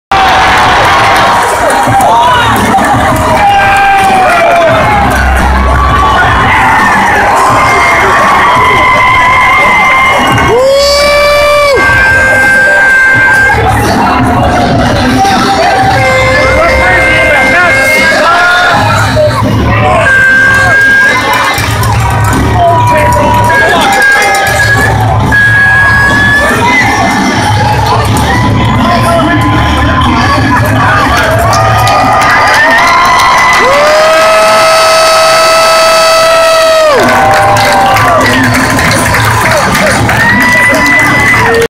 Grand Rapids Griffins vs. Checkers Fight 2/20/16
Mitch Callahan vs. Brock Mcginn